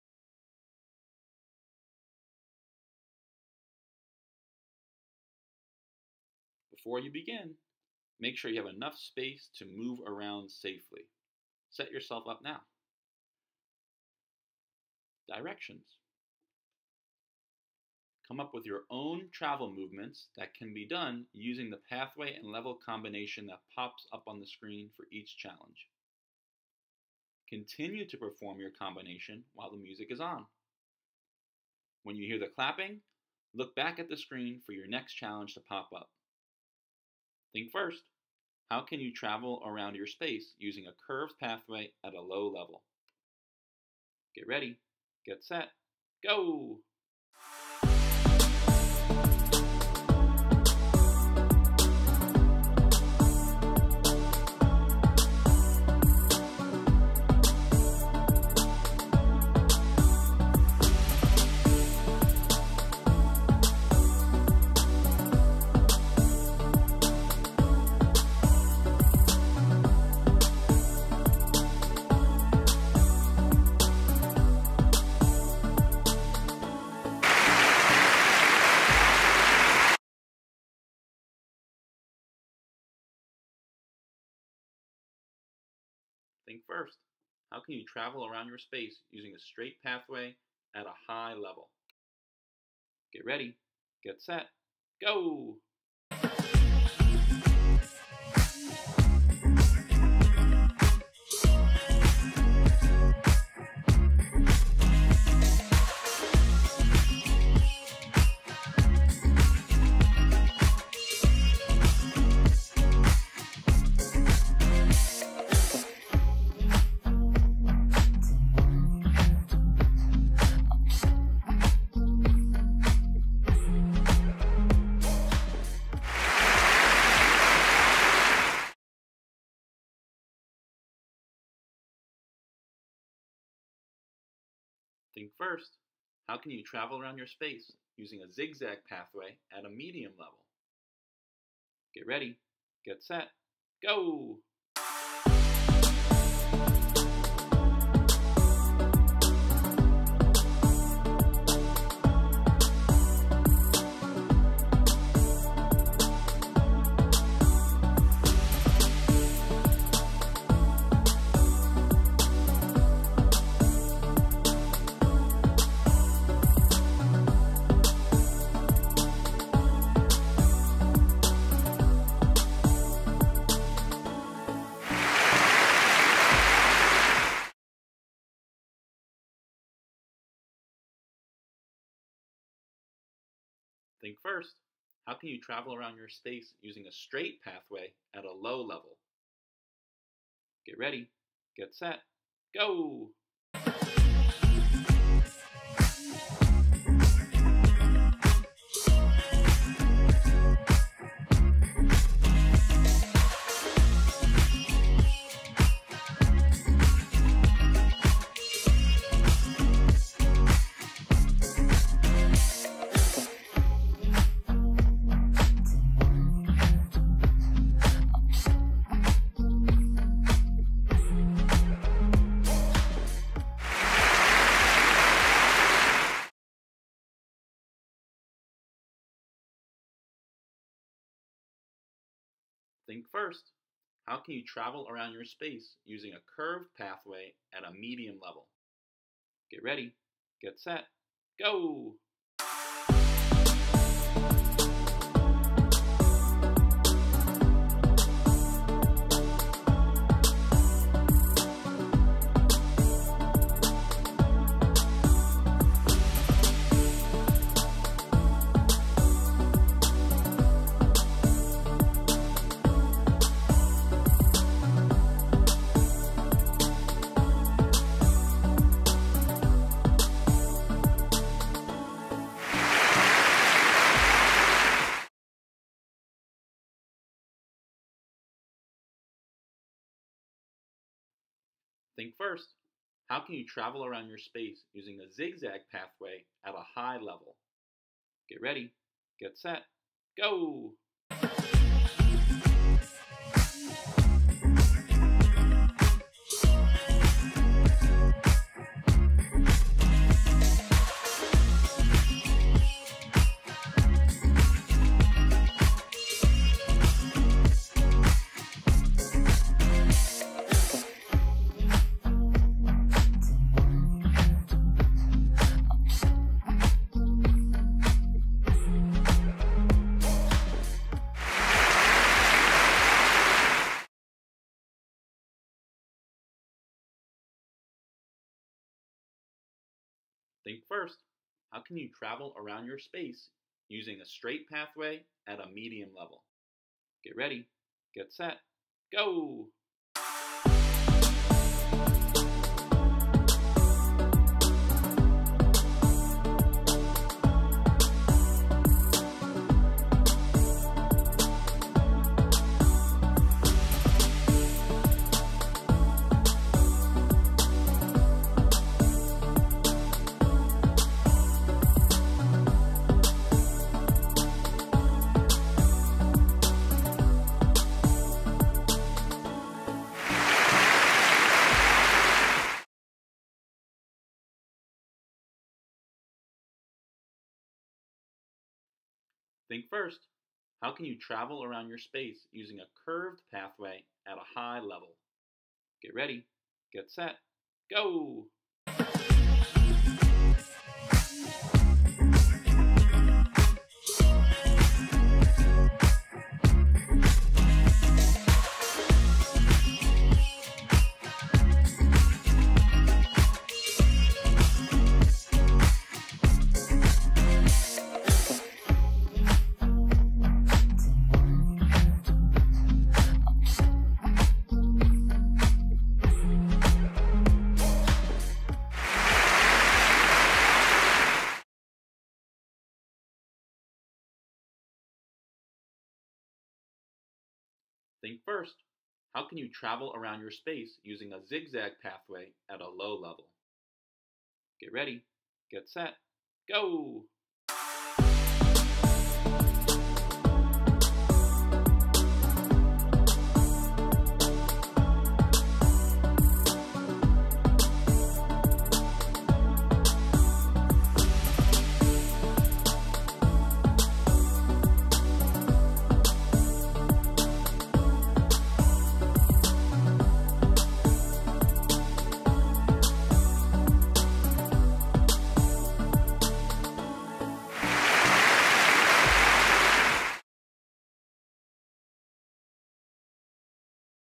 At-Home P.E. - Pathway/Level Combination Exploration!
Use this video for students' at-home P.E. instruction as they expand on their exploration of pathways (zig-zag, curved and straight) and levels (low, medium and high) by combining these concepts with travel movements of their choice to complete various challenges!